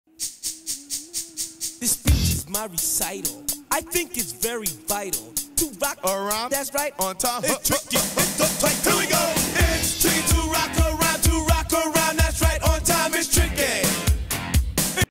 Boots boogie in ! Black spiny mouse!
Here we have the spiny mouse boogie with Boots!! A rarely seen rodent in my collection of pets. I finally caught her out of the cage and easy to video. As some of you have seen, she is about 10x as fast as any of the chipmunks. I hope you enjoy the song and the video.